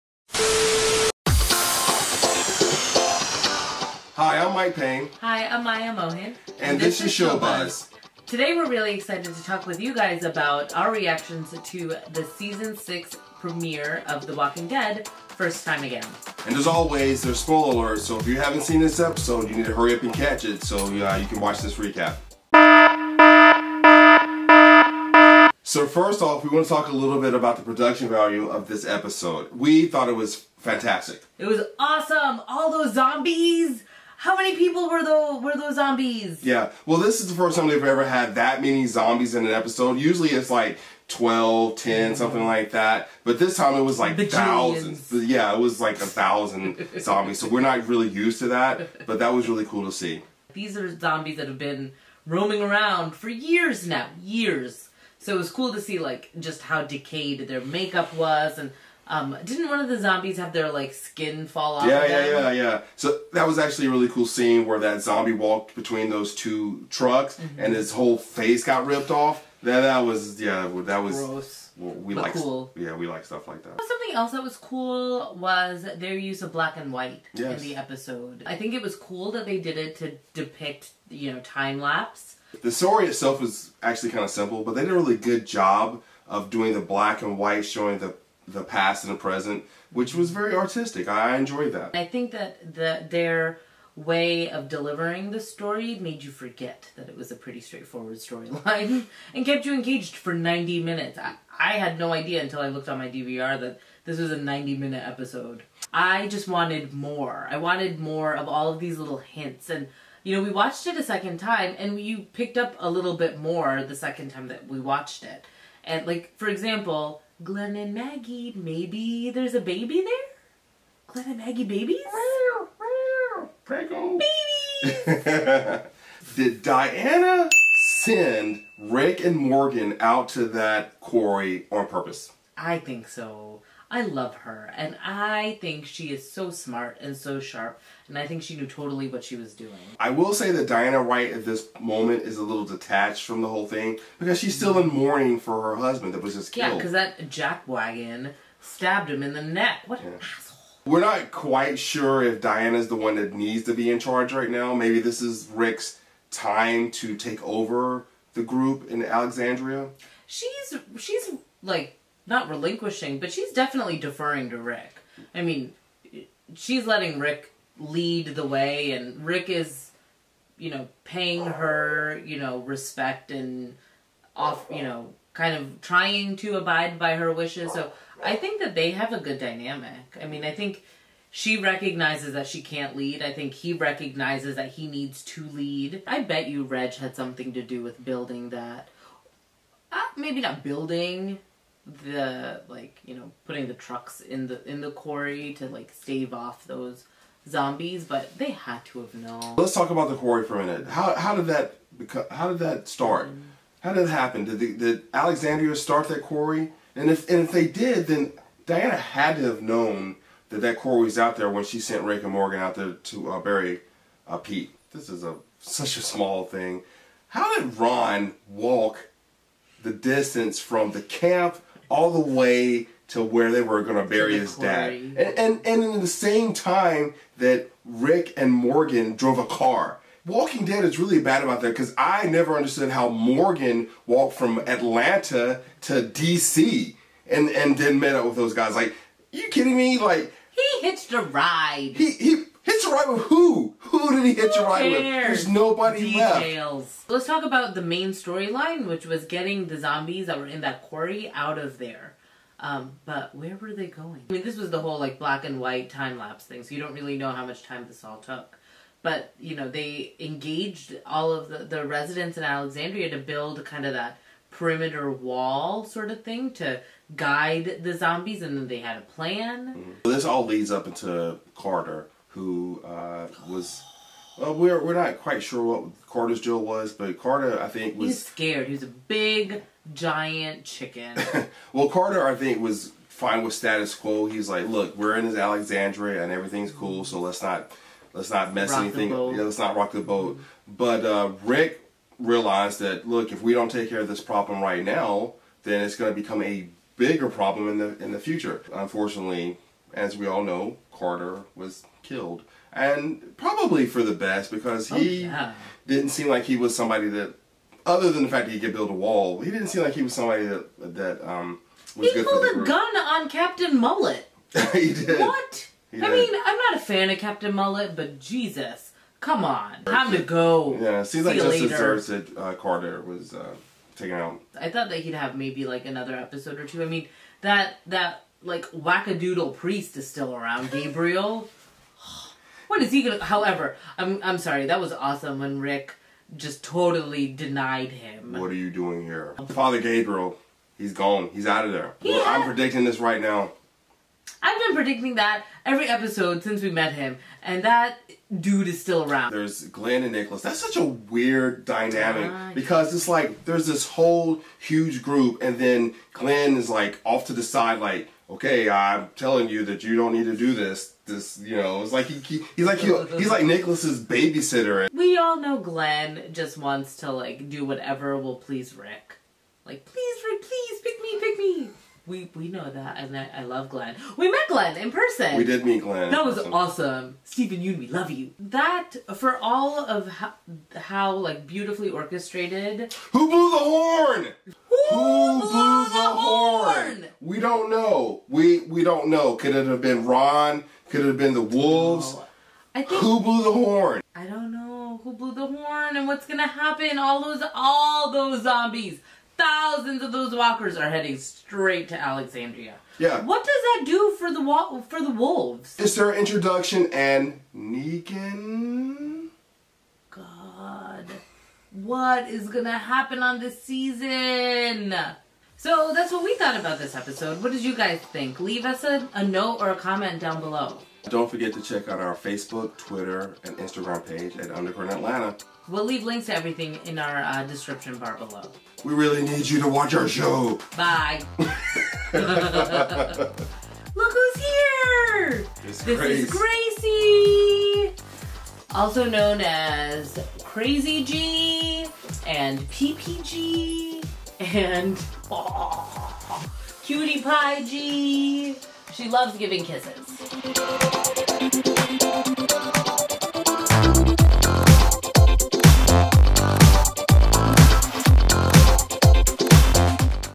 Show Buzz Episode 1 "Who Blew The Horn"
Welcome to Show Buzz,  a weekly recap of our favorite television shows. We needed to share our reactions to these shows with more than just each other and our pets, so we created this show to engage with a larger audience.

Right now, we are focusing on one of our favorites, The Walking Dead, with our first episode "Who Blew the Horn".

Definitely leave us a comment to let us know what you thought of the episode.